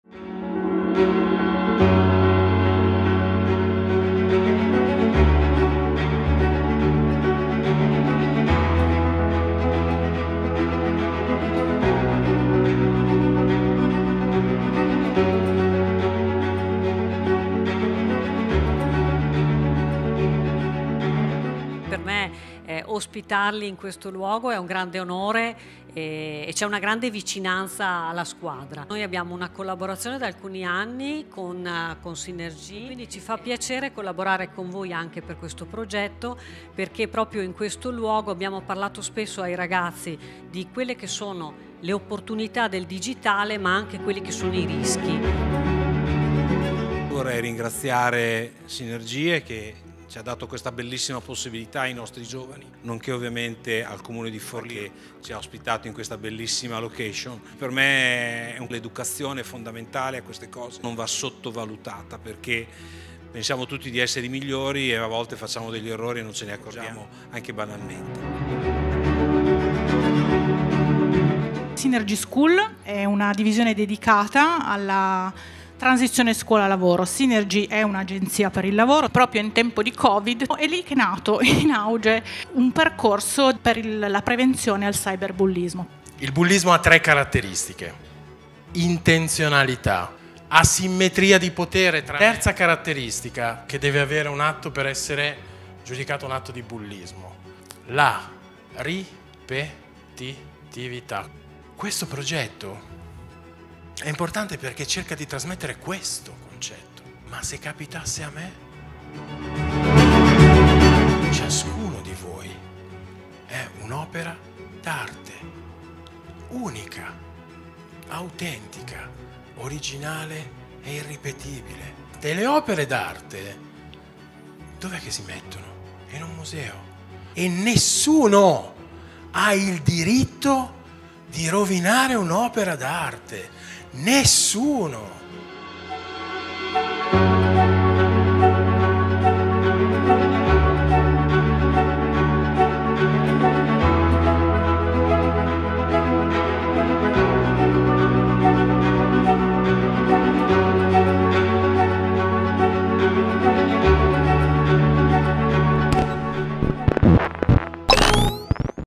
Lezione aperta "Bullismo? Ti Rimbalzo"
👀 Guarda il video della serata e scopri com'è andata!

🤝🏻 Grazie ancora al nostro partner @synergieitalia che, insieme alla @fondazione_carolina e @risorseitaliasrlnazionale , hanno portato avanti la lotta contro il bullismo online con la presenza di oltre 120 persone tra cui il settore giovanile della Pallacanestro Forlì, amici e partner.

📹Riccardo Lolli